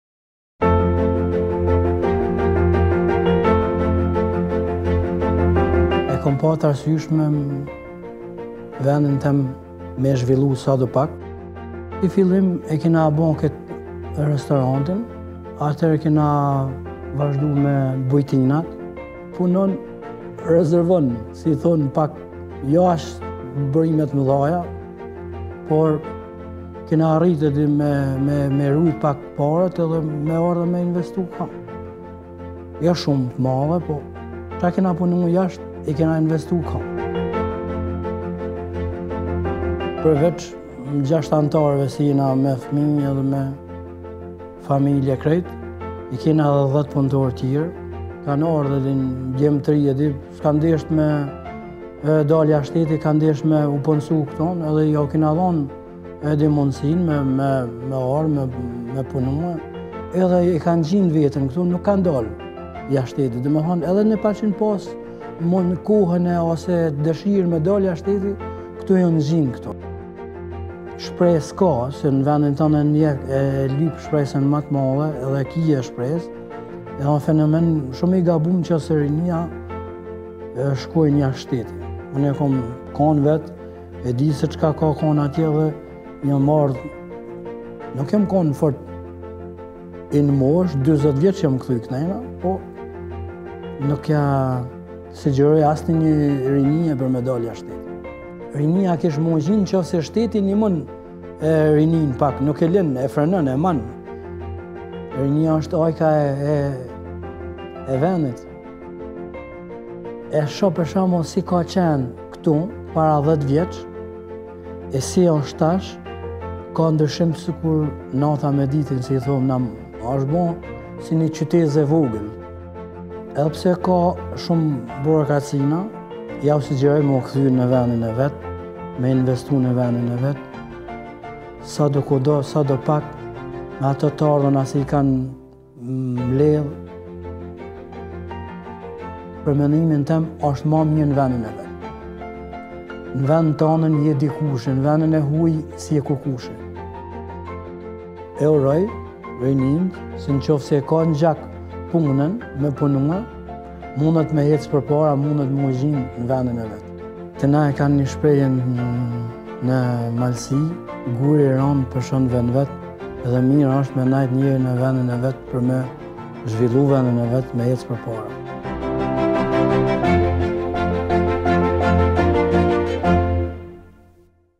Ardian Selimi - Retrunee - Tropoje
Ardian Selimi është një i kthyer nga emigracioni, i cili pas viteve të kaluara në Kanada, ka vendosur të kthehet për të investuar në Valbonë, Tropojë. Ardiani ka zgjedhur ta ndërtojë të ardhmen në vendin e tij. Si fillim, ai ka hapur restorantin e më pas bujtinën, duke krijuar një nga pikat më tërheqëse në Valbonë, @valbona.relax . Përveç familjes së tij, ai ka krijuar mundësi punësimi edhe për 10 punëtorë të tjerë, duke kontribuar në zhvillimin e zonës me këtë sipërmarrje.

Ju ftojmë të ndiqni historinë dhe mesazhet e bukura të Ardianit.

📌 “Perspektiva të Reja” zbatohet nga Swisscontact në bashkëpunim me TAG International, dhe financohet nga Qeveria Britanike nëpërmjet Ambasadës Britanike në Tiranë.